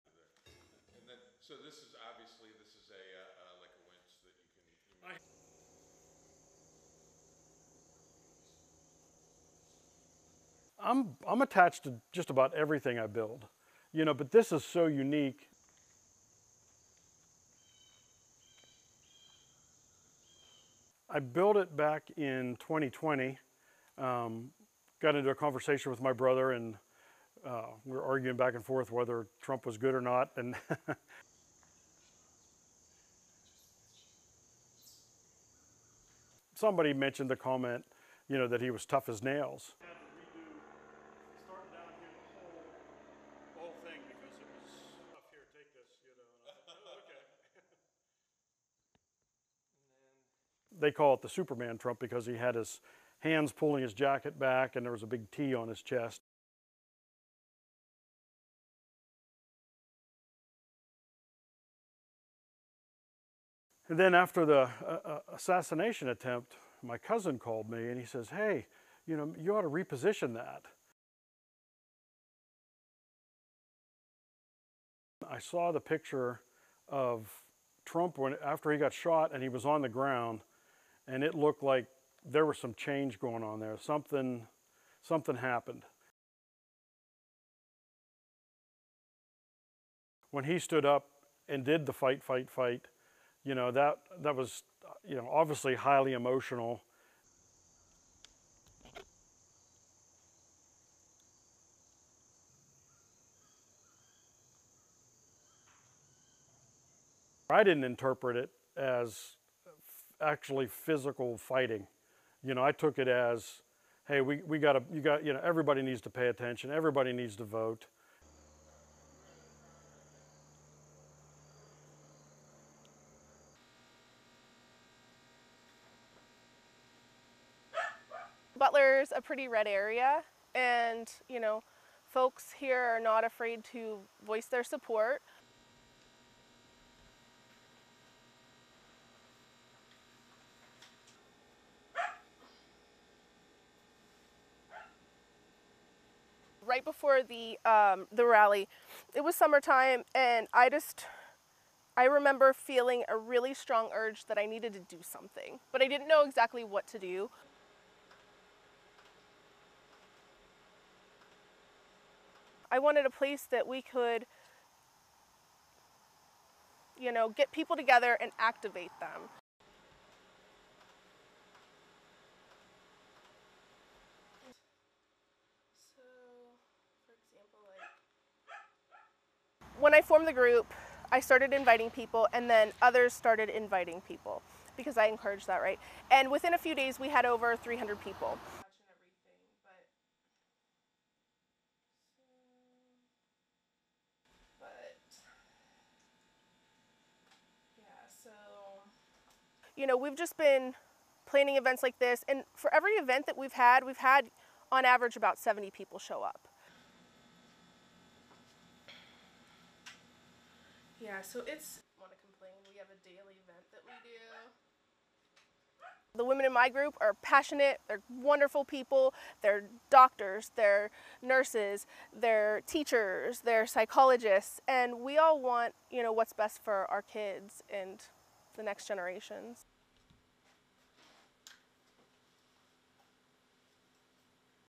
Voters in Pennsylvania town encourage others to vote, participate in politics after Trump shooting
RESTRICTION SUMMARY:

ASSOCIATED PRESS
Butler, Pennsylvania – 26 September 2024
1. Various of ‘Tough as Nails’ statue of Donald Trump
2. SOUNDBITE (English) Bill Secunda, Metal Artist:
“I'm attached to just about everything I build, you know? But this is so unique. And in it, you know, it was the circumstances were unique.”
3. Tight of ‘Tough as Nails’
4. SOUNDBITE (English) Bill Secunda, Metal Artist:
“I built it back in 2020. Got into a conversation with my brother, and we're arguing back and forth whether Trump was good or not."
5. Wide of statue
6. SOUNDBITE (English) Bill Secunda, Metal Artist:
“Somebody mentioned the comment, you know, that he was tough as nails.”
7. Various of Secunda by the statue explaining his work
8. SOUNDBITE (English) Bill Secunda, Metal Artist:
“They call it the ‘Superman Trump’ because he had his hands pulling his jacket back and he had a big ‘T’ on his chest.”

ASSOCIATED PRESS
ARCHIVE: Butler, Pennsylvania – 13 July 2024
9. STILL of Trump raising his fist in the air after being shot at

ASSOCIATED PRESS
10. SOUNDBITE (English) Bill Secunda, Metal Artist:
“And then after the assassination attempt, my cousin called me and he says, ‘hey, you know, you ought to reposition that.’”

ASSOCIATED PRESS
11. STILL of Trump raising his fist in the air after being shot at

ASSOCIATED PRESS
12. SOUNDBITE (English) Bill Secunda, Metal Artist:
“I saw the picture of Trump after he got shot and he was on the ground, and it looked like there was some change going on there or something. Something happened.”

ASSOCIATED PRESS
13. STILL of Trump raising his fist in the air while being pushed into car by Secret Service Agents

ASSOCIATED PRESS
14. SOUNDBITE (English) Bill Secunda, Metal Artist:
“When he stood up and did the ‘fight, fight, fight,’ you know, that, that was, you know, obviously highly emotional.”
15. Various of statue
16. SOUNDBITE (English) Bill Secunda, Metal Artist:
“I didn't interpret it as actually physical fighting, you know, I took it as, ‘hey, we gotta, you got, you know, everybody needs to pay attention. Everybody needs to vote.”
17. Medium of statue

ASSOCIATED PRESS
Butler, Pennsylvania – 27 September 2024
18. Wide of Heidi Priest sitting at table on laptop
19. SOUNDBITE (English) Heidi Priest, Butler Resident:
“Butler's a pretty red area, and, you know, folks here are not afraid to voice their support.”
20. Tight of Priest working
21. Tight of painted rocks in Priest’s kitchen
22. SOUNDBITE (English) Heidi Priest, Butler Resident:
“Right before the, the rally, it was summertime. And I just I remember feeling a really strong urge that I needed to do something, but I didn't know exactly what to do.”
23. Medium of Priest on laptop
24. SOUNDBITE (English) Heidi Priest, Butler Resident:
“I wanted a place that we could, you know, get people together and activate them.”
25. Various of Priest on laptop
26. SOUNDBITE (English) Heidi Priest, Butler Resident:
“When I formed the group, I started inviting people and then others started inviting people because I encouraged that, right? And within a few days, we had over 300 people.”
27. Various of Priest on laptop
28. SOUNDBITE (English) Heidi Priest, Butler Resident:
“You know, we've just been planning events like this, and for every event that we've had, we've had on average about 70 people show up.”
STORYLINE: